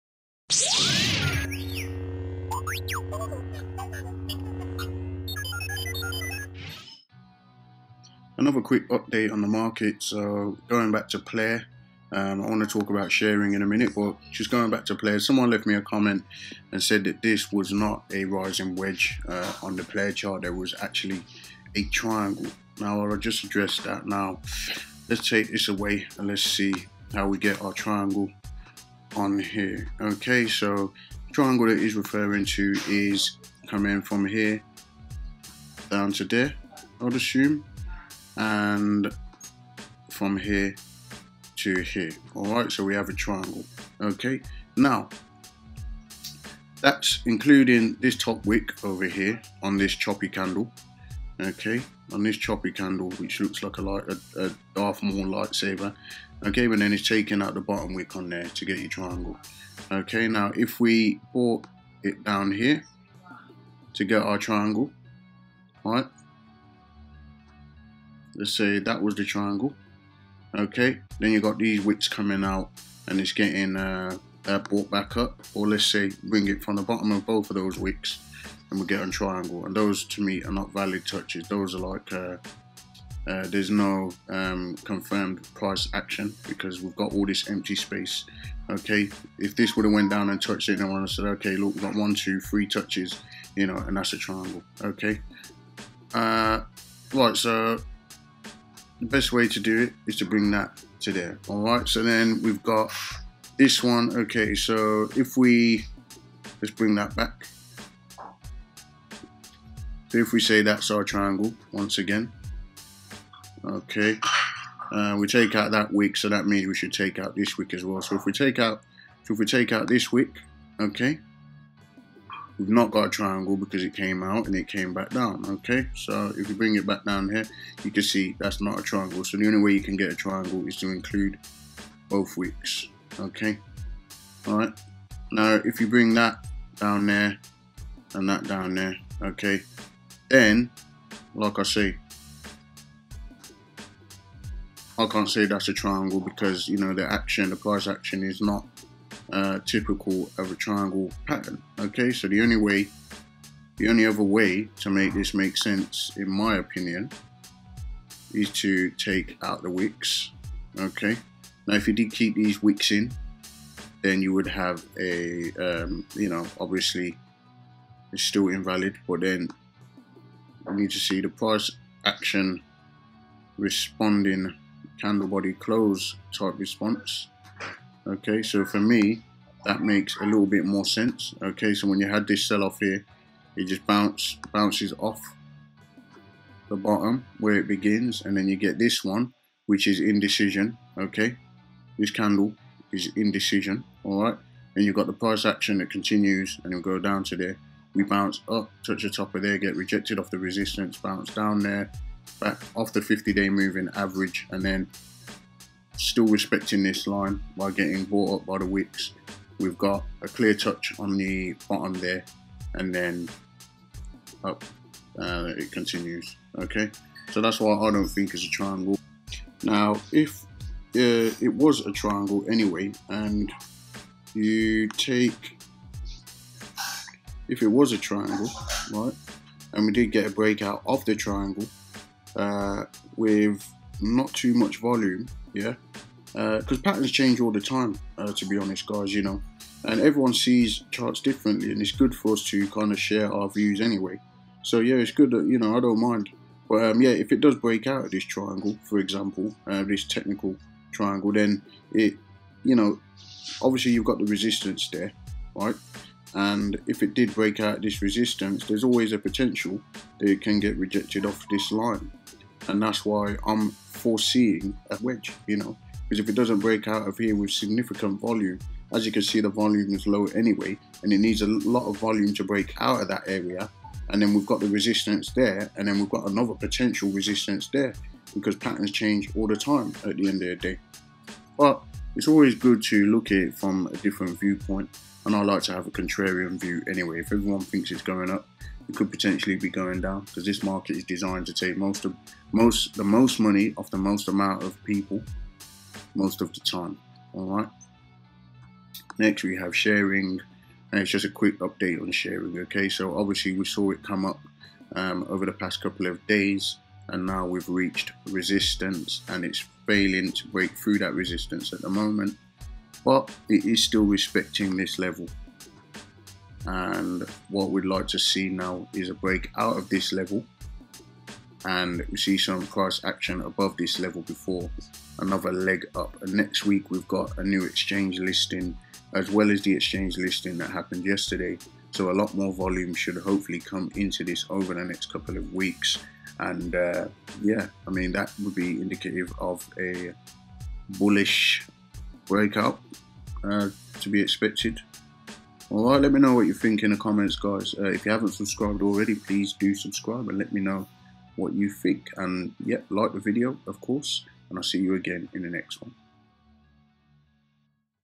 Plair PLA Triangle Or Rising Wedge? | Hidden Gem Sharering SHR Meets Resistance | Breakout Coming?
A quick update from my previous video. In this video I cover my recent technical analysis on Crypto Hidden Gem Plair (PLA) and also a brief update on Sharering (SHR) which is also one of my hidden Gems. Could there be a breakout coming soon or both projects? Only time will tell.

I do not expect them, but donations are always accepted and appreciated:
My addresses -
BTC: 3A9EcsoaZQquiu4uVGQojXbE84VHGrDnp8
ETH: 0x789315ae0c3f32ae6a4a38c01dac4054282fbf61